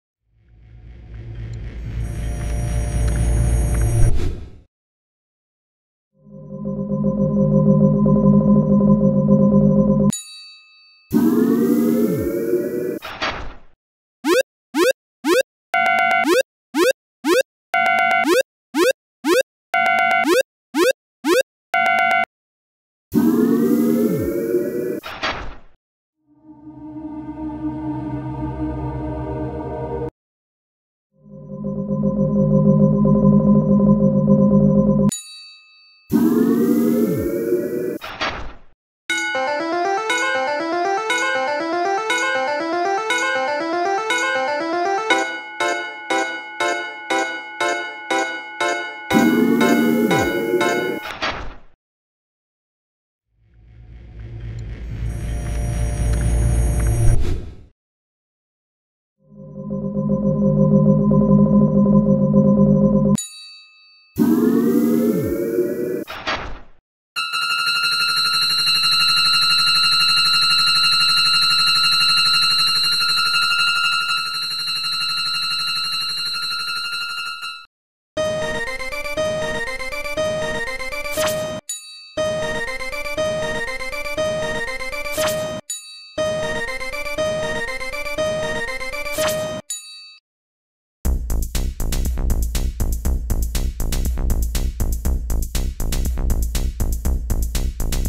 エレベーターの扉が開いて踏切がカンカン
通路をすすむその先にはエレベーター。そのエレベーターの扉が開くと、なんと変な踏切が奇妙な動作をしてカンカンする動画